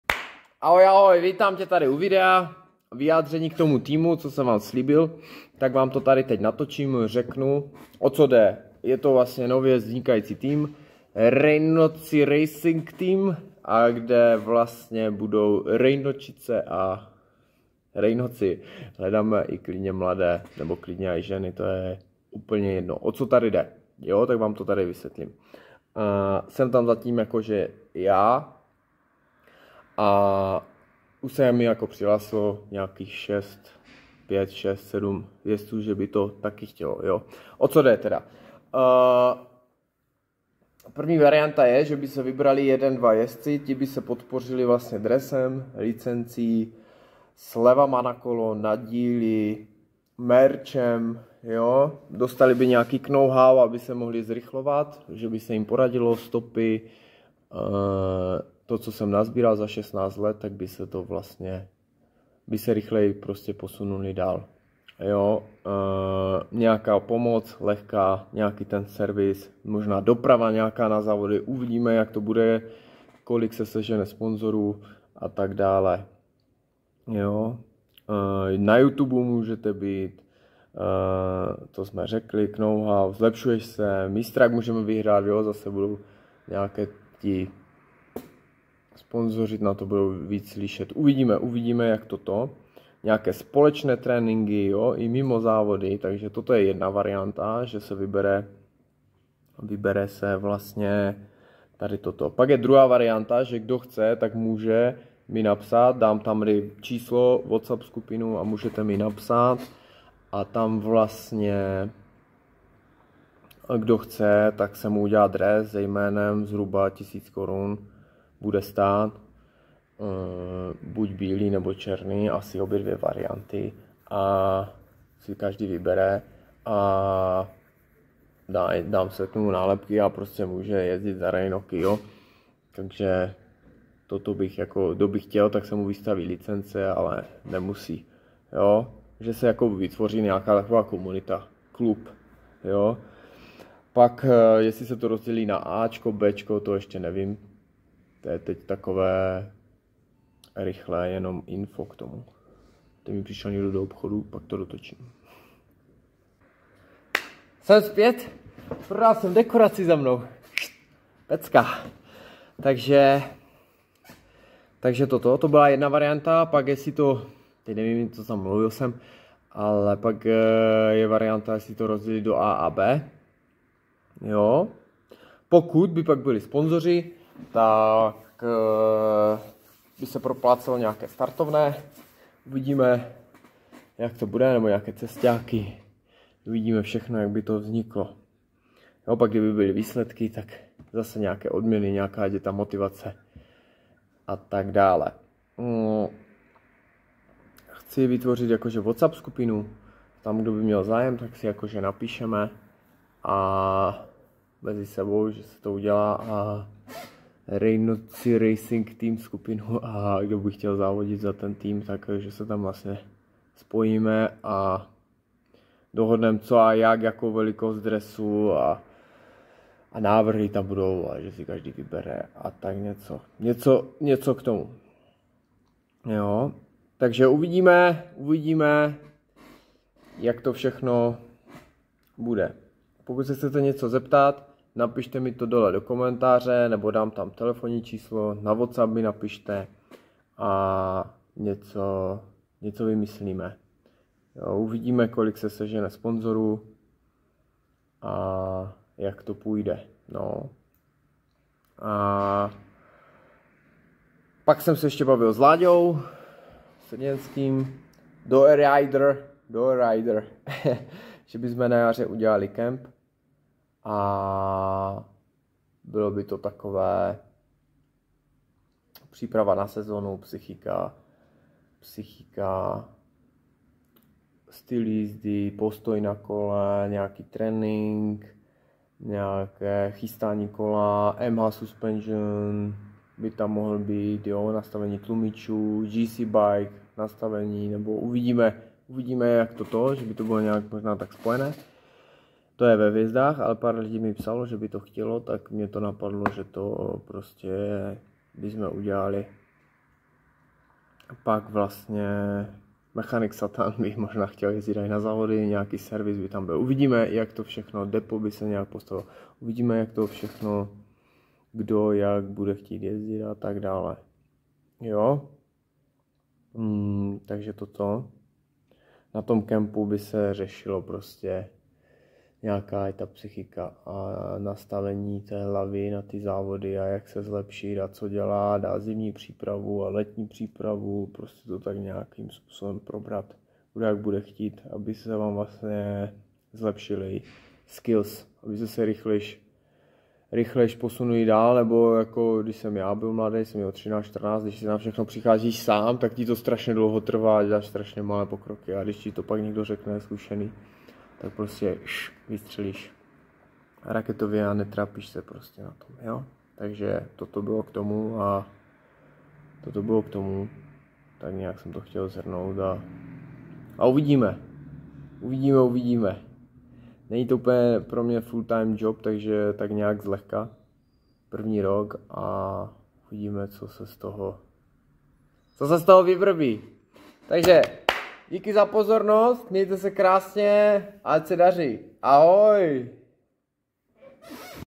Rejnoci racing team / Informace k nově vznikajícímu teamu / Rejnoci na tripu
Ahoj rychlé info k týmu. Do týmu může každý jak enduro, motocross, xc. Zde mi můžete psát na wats up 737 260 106 kde bude více info a budeme ladit dresy velikosti atd.

Slevový kod - 20% REJNOK na nábytek do 15. listopadu nabídka nábytku na Případně posílám :*